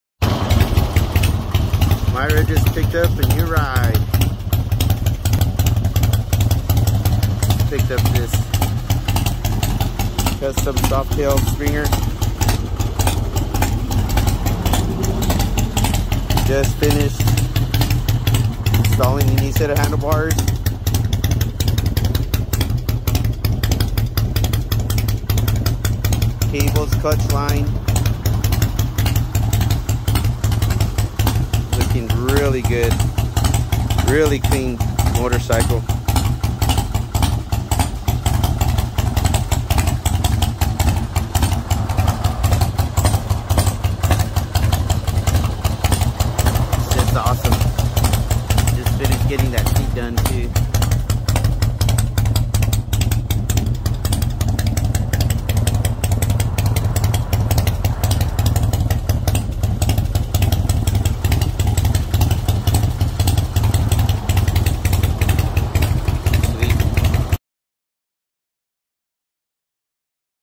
Mayra's Custom Springer Softail
Mayra's sweet Custom Springer Softail. We installed a new set of handlebars w/ all stainless steel lines, new internal wiring, grips. Rerouted front and rear brake lines. What an awesome and super clean Springer Softail!